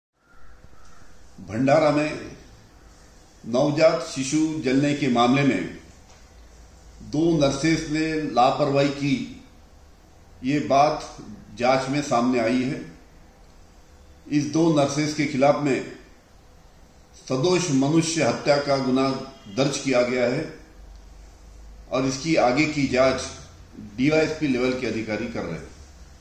Maharashtra k Bhandara me 10 navjaat bachcho ki maut pr kya hui karwai Anil deshmukh HM.
byte .anil deshmukh home minister maharashtra

FIR has been registered in PS Bhandara.CR no is 52/21 u/ s 304 part 2 .
Complainant - Arun Waykar SDPO Sakoli
Accused - Shubhangi Sathavane - Nurse
2 Smita Ambildhuke - Nurse .
About the criminal negligence on the part of Nurses in the death of the 10 infants in Bhandara for their liabilities after the enquiries in the investigation of Accidental death.
The investigation is under progress.
For information.
Hemant Nagrale
DGP Maharashtra State